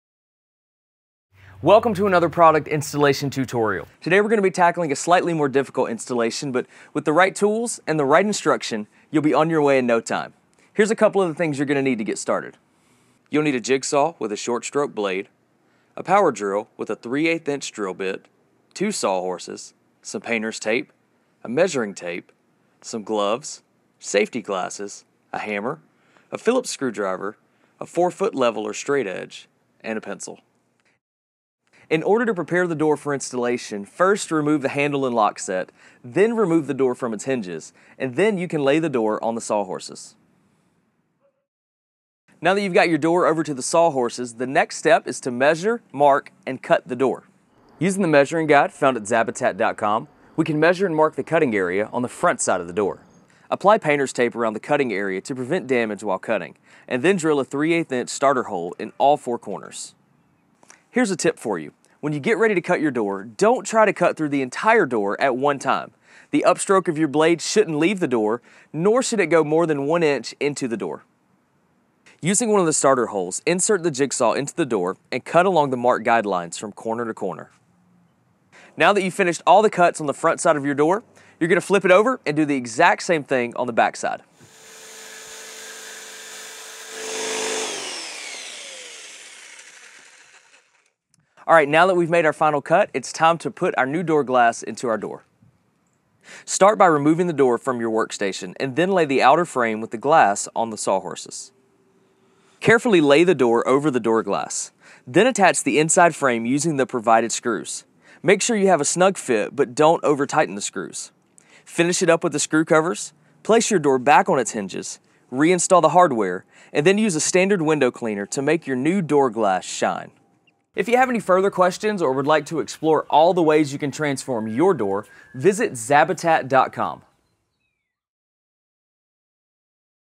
How to Add Door Glass to Your Door - Door Glass Installation Guide
In this product installation tutorial video, we are adding a standard full door light with ODL decorative door glass fromzabitat.com to dress up your front door and add some curb appeal to your home. This DIY project shouldn't take more than an afternoon to complete!